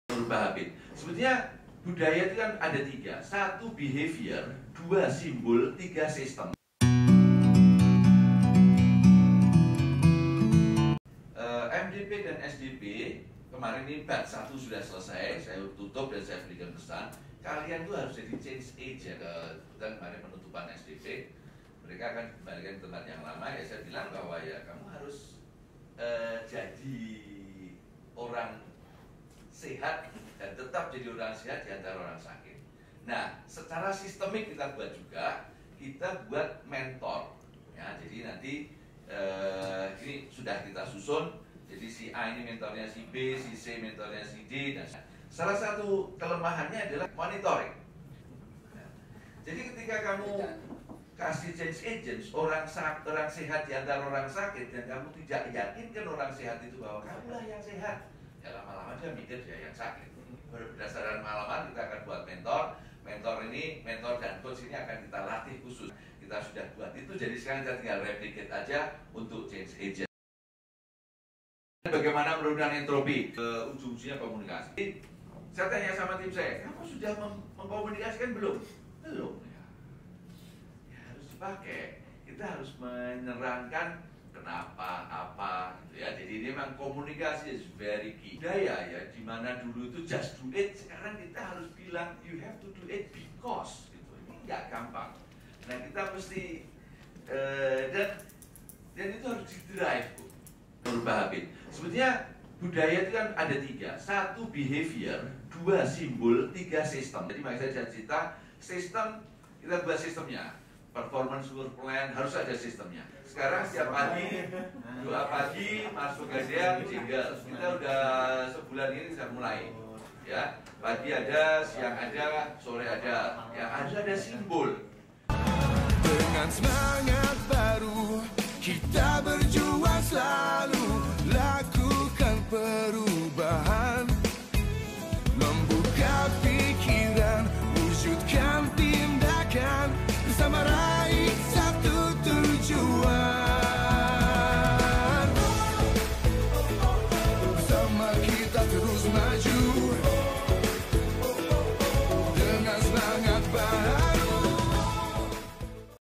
Presentasi M.Edi Isdwiarto (Dir SDM & Hukum )saat menjadi narasumber di Acara Transformasi Manajemen
Dalam seminar yang diadakan oleh intipesan yang berthema Transformasi Manajemen dan subthema Strategi dan Langkah-langkah membuat organisasi semakin gesit dan siap menghadapi perubahan zaman